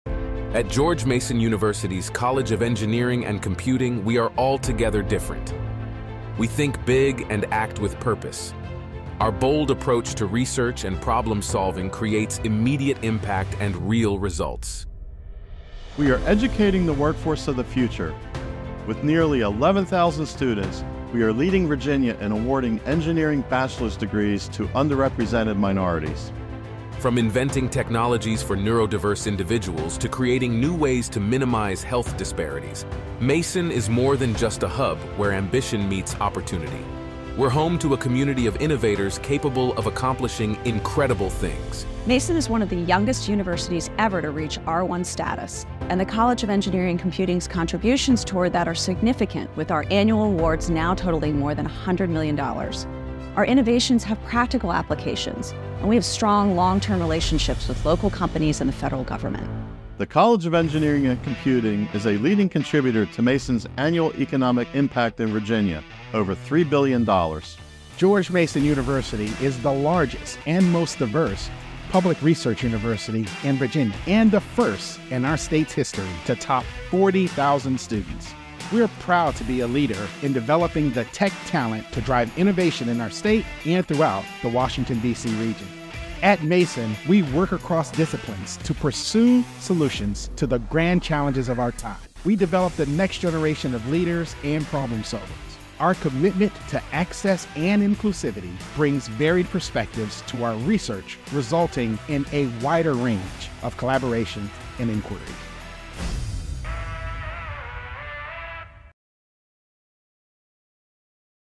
George Mason University's College of Engineering and Computing
As the most established university in Northern Virginia, we have strong ties to industry and government in the Washington, D.C., area. With convenient access to high-tech spaces and expanding facilities, students experience a choice of majors and minors within any of the ten academic departments housed in the College of Engineering and Computing. We offer a dynamic education that combines academic instruction with real-world applications that will prepare our students for success.